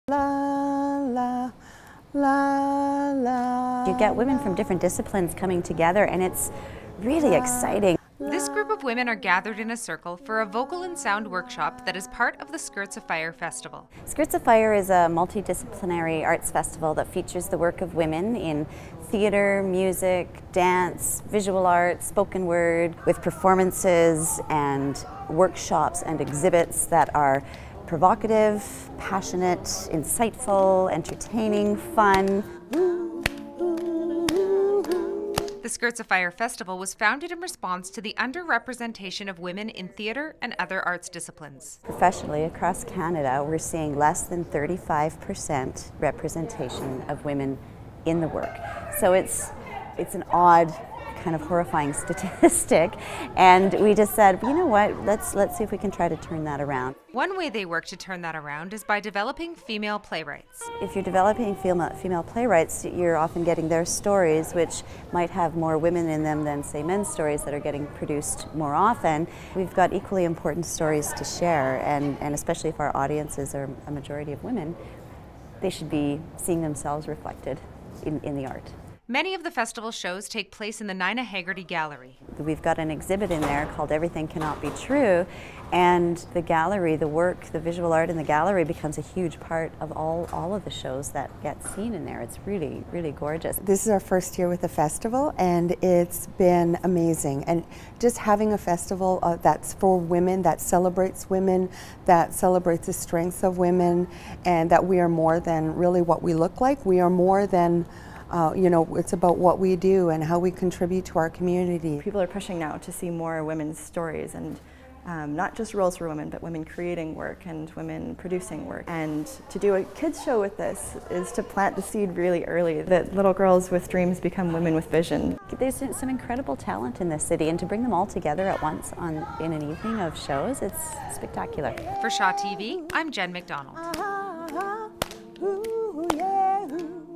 Skirts a Fire
This local festival explores the inspiring stories of women through dance, theatre, music and art. Shaw TVís Jen McDonald takes at look at the skirts-a-fire festival @SkirtsAFire